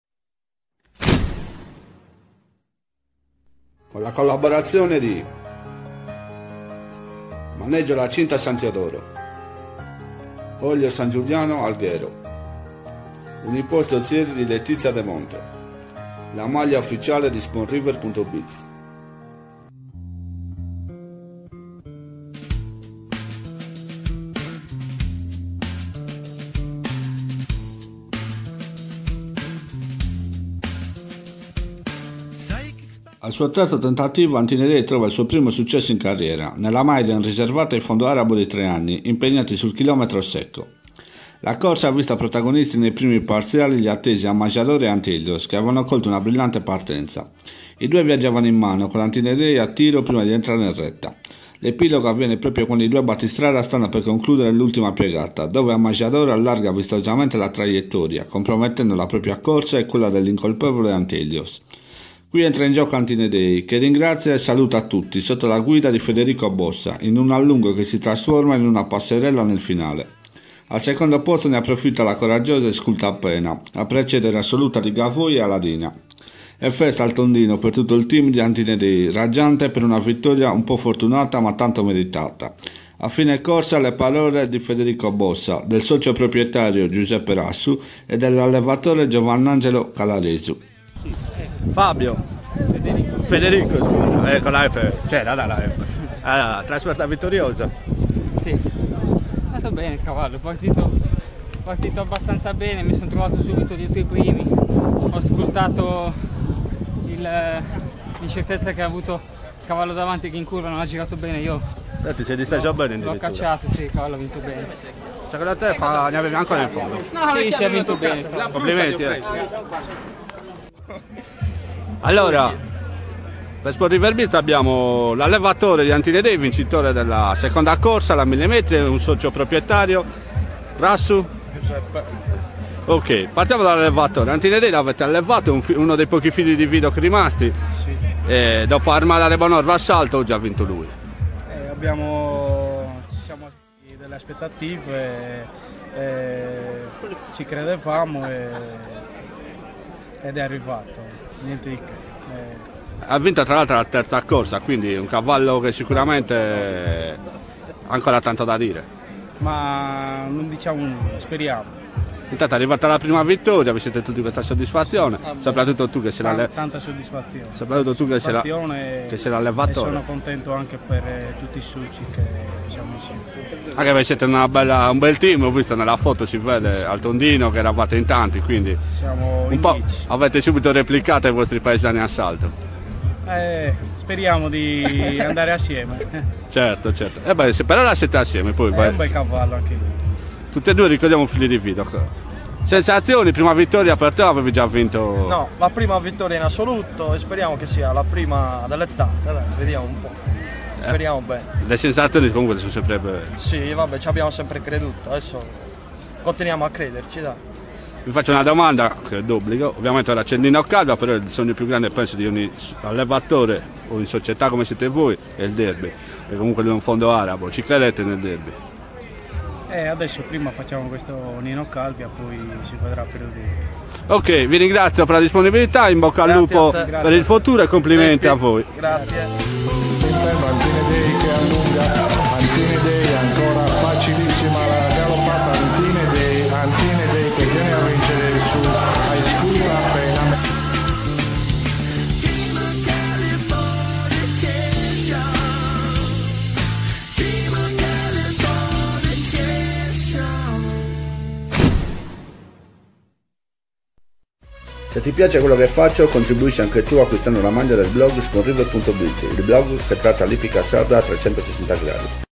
La "prima" in carriera per il Fondo Arabo Antine Day, interviste a Federico Bossa e ai proprietari
19/07/2019 - Ippodromo di Sassari - 2° Corsa
La clip video che documenta la prima vittoria in carriera per il Fondo Arabo di 3 anni Antine Day, con in sella Federico Bossa. Nel video le interviste fatte nel post corsa al fantino Federico Bossa, al socio-proprietario Giuseppe Rassu e dell'allevatore Giovannangelo Calaresu.
Ordine d'arrivo:
1° Antine Day - Federico Bossa
2° Aisculta Appena - Maikol Arras
3° Assoluta di Gavoi - Luca Etzi
4° Aladina - Pasquale Salis